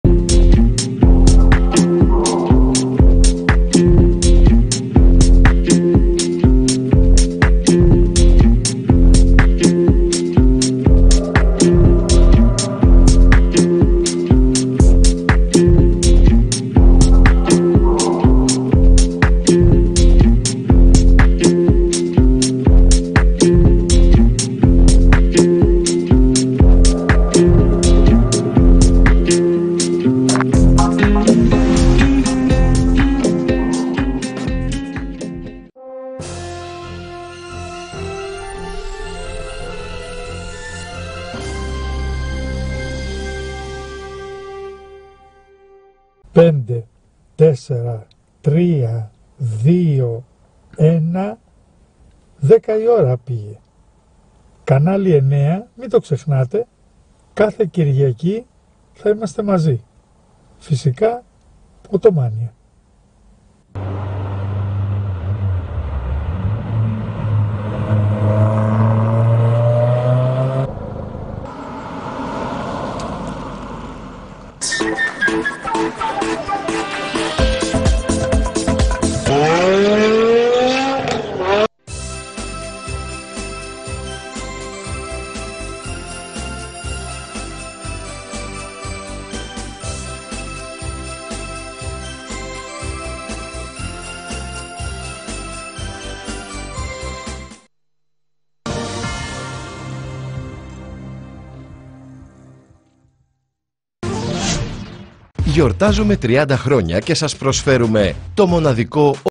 Channel 9 2021-12-25-23-40-07
Καταγραφή του Channel 9 την ημέρα των Χριστουγέννων.

Περιέχει:

⚬ Channel 9 (Greece) Ident 2005-σήμερα
⚬ Channel 9 (Greece) Christmas ident 2005-σήμερα
⚬ Channel 9 (Greece) Ακολουθεί εκπομπή τηλεπώλησης ident 2013-σήμερα